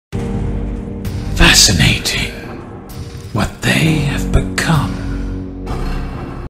FNAF SFM | Afton Voice (Preview)
hello guys welcome to my second animation project that is coming soon. i thought making a voiceline animation, is gonna help me to improve my animation style. anyway see you guys next time!

I'm very sorry for the preview delay.